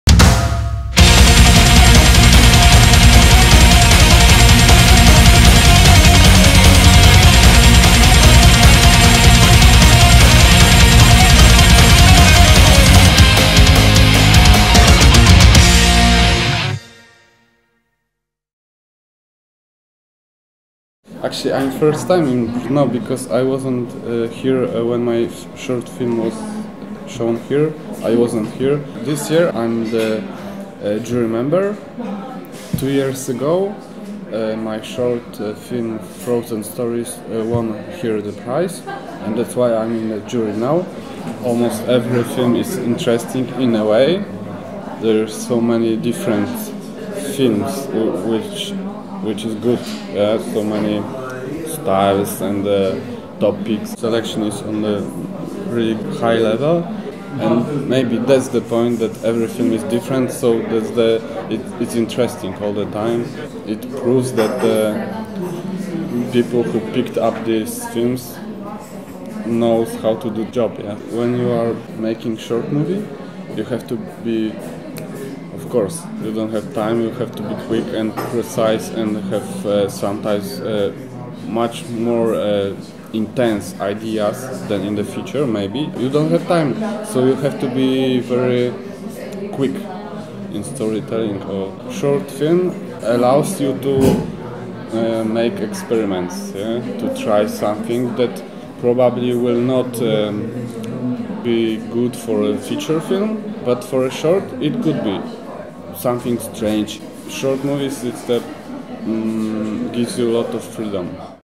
Interview with Grzegorz Jaroszuk
Member of jury B16 and winner of competition in 2012